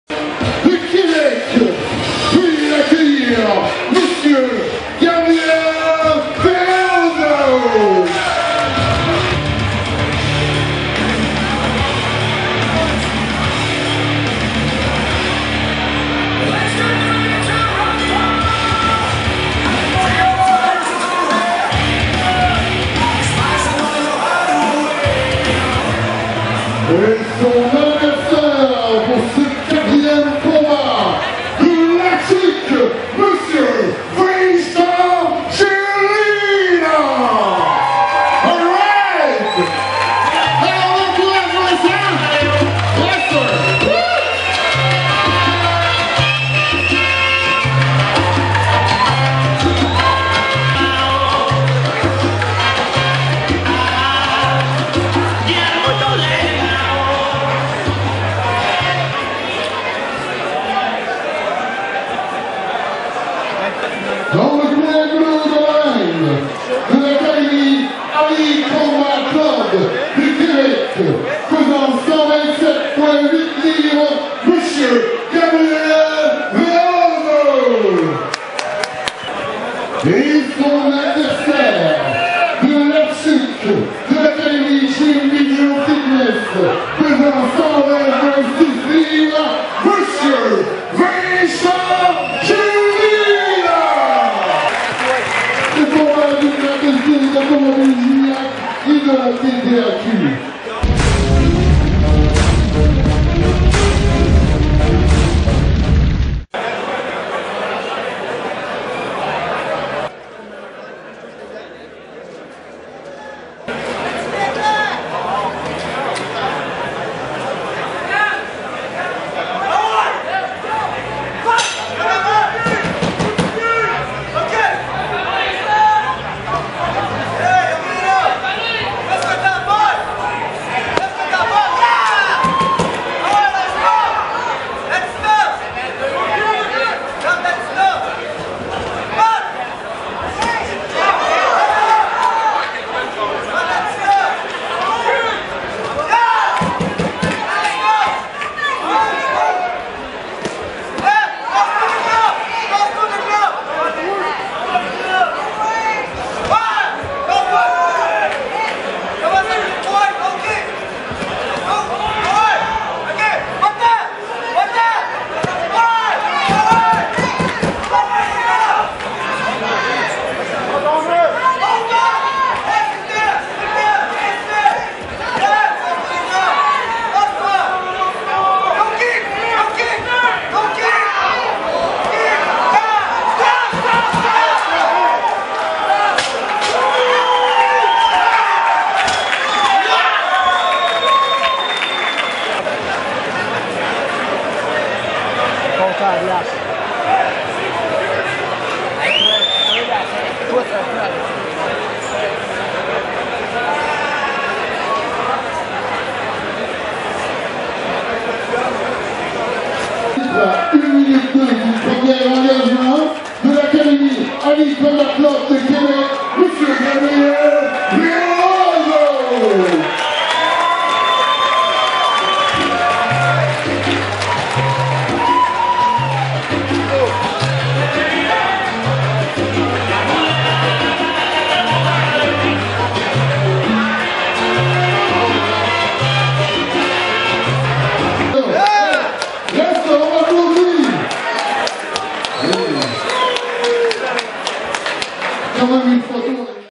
Vincent Gélinas VS Gabriel Velozo
Vincent Gélinas affronte Gabriel Velozo lors du GALA MMA amateur BIG BANG LA GUERRE DU FEU qui s'est tenu à La Tuque, Québec, Canada le 10 août 2013